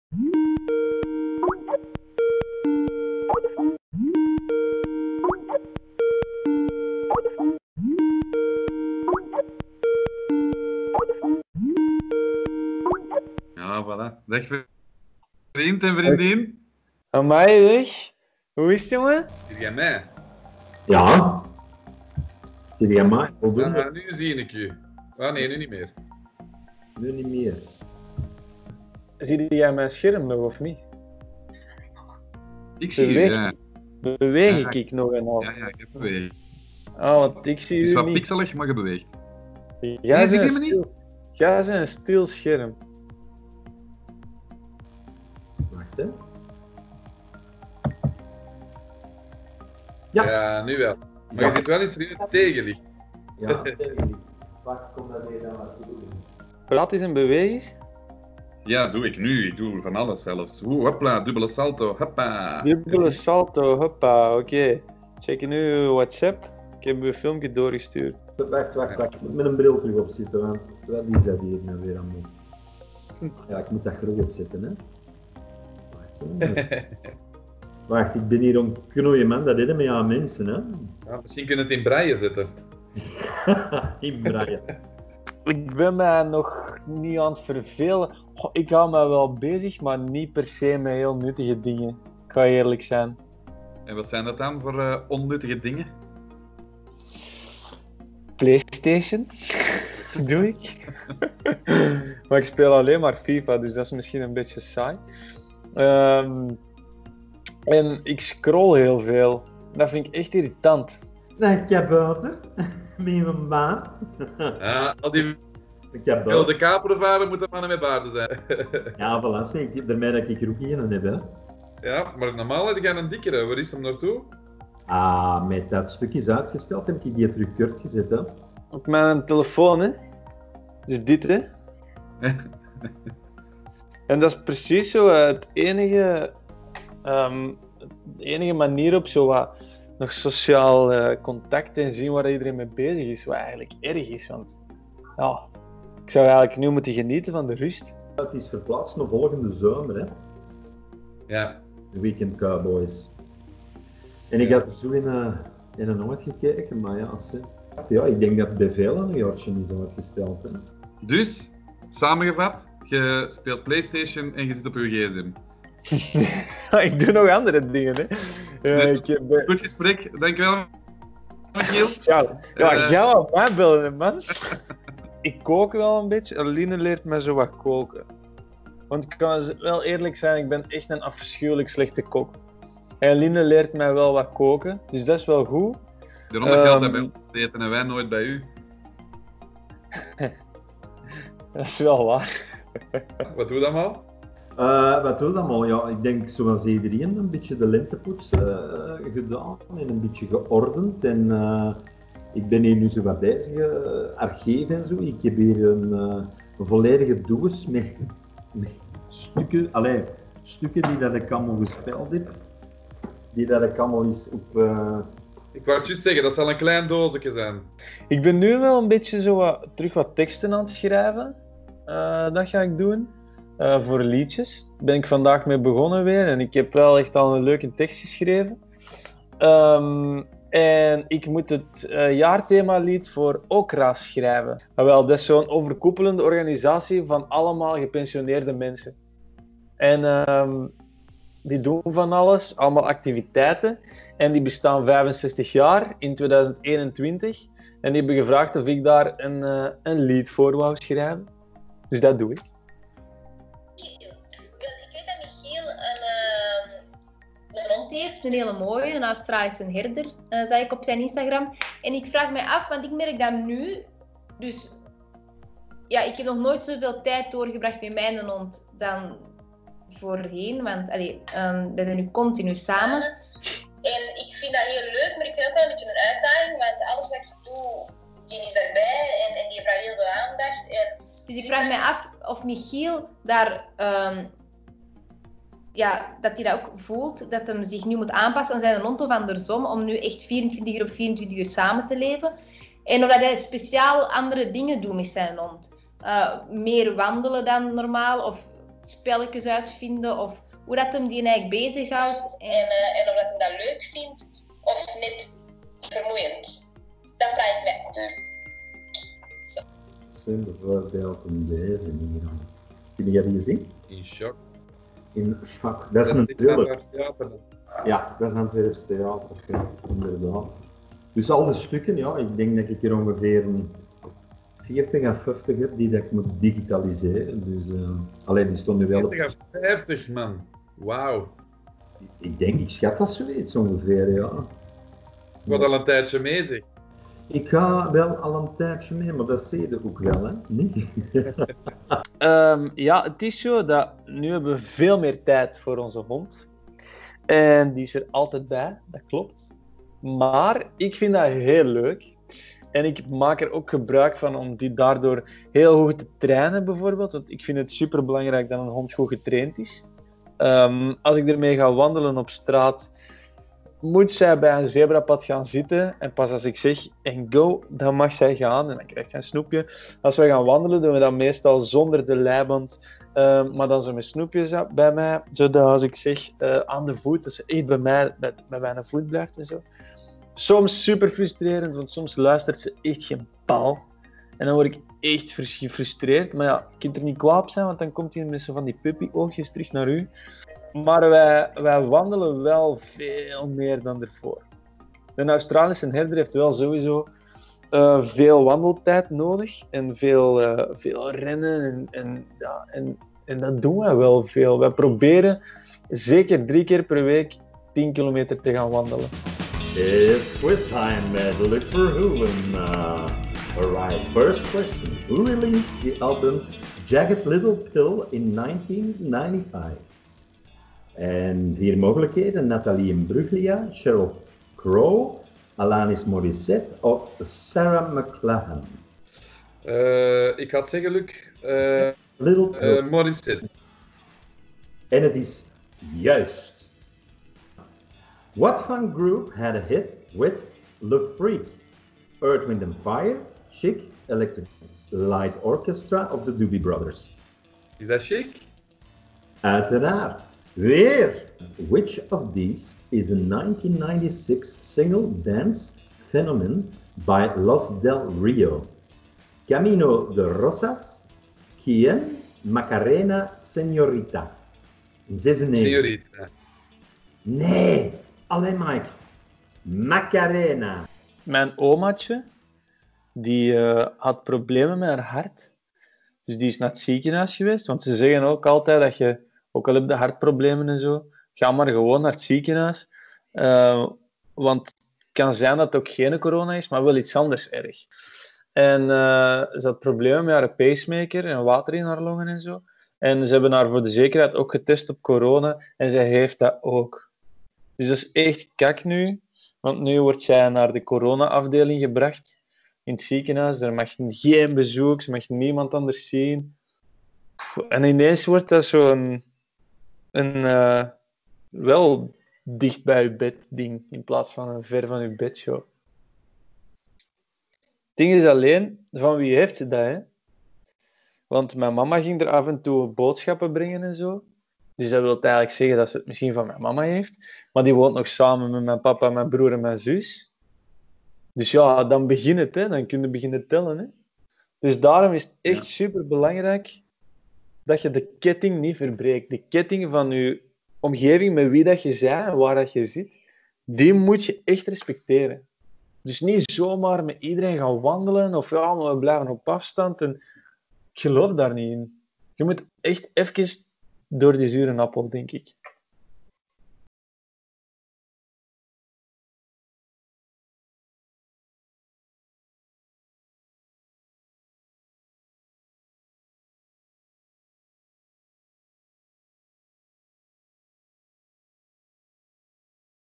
Corona gesprekken, Michiel De Meyer & Luc Verhoeven
In gesprek met inspirerende mensen over de corona crisis in België. Wat als de verveling toeslaat? Dan ga je toch gewoon wandelen met de hond of je speelt een quiz. In deze derde aflevering spreek ik met spring-in-'t-veld Michiel De Meyer en gevestigde waarde Luc Verhoeven.